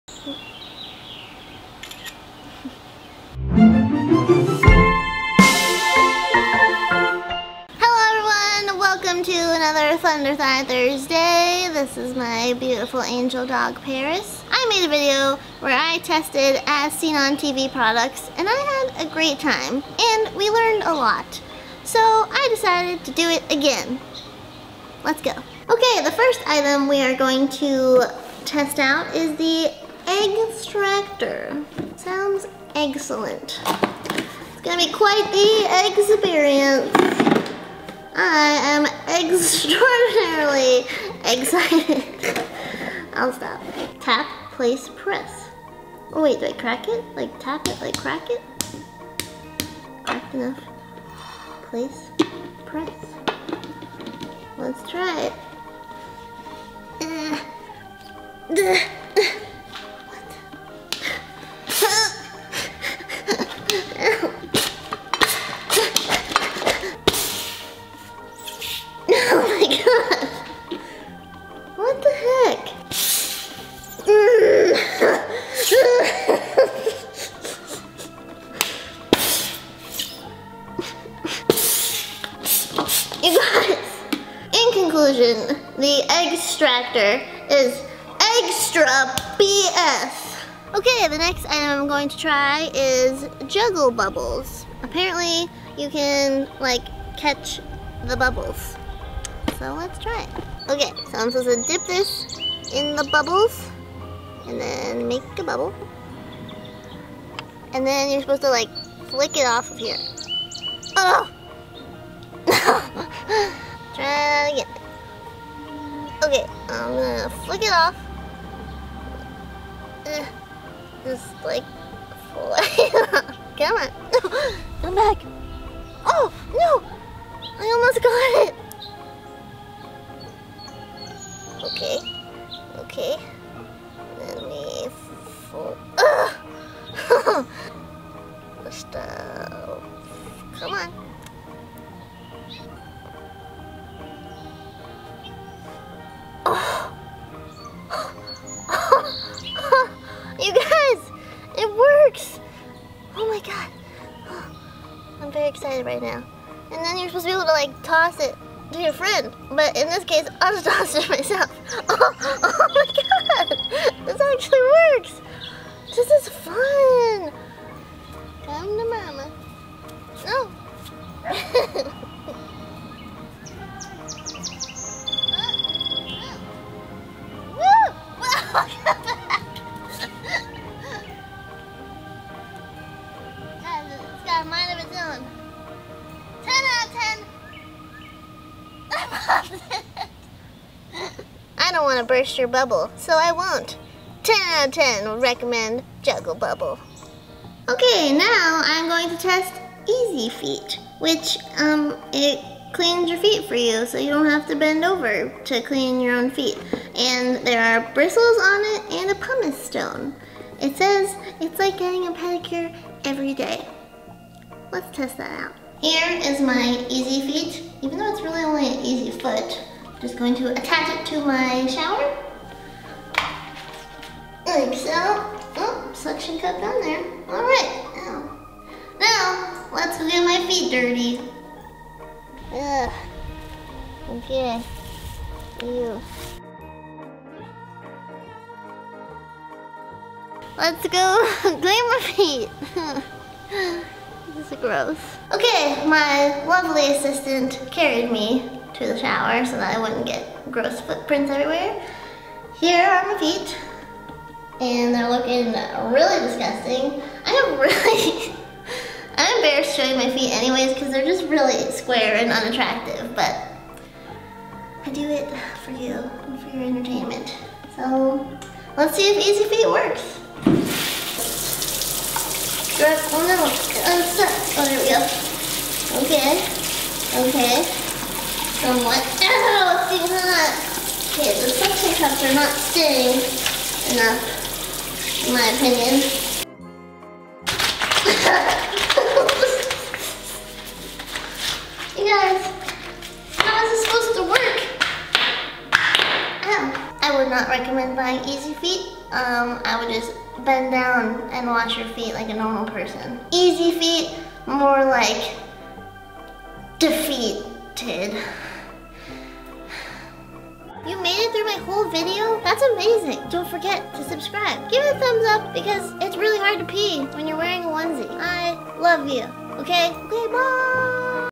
BUBBLES & SHOWERS | Meghan McCarthy
I tested more As Seen on TV Products so next time you want to buy some dumb thing off an infomercial, watch this video first to make sure it works! K LOVE YOU BYE.

★★★ADD ME ON SNAPCHAT★★★ MWMM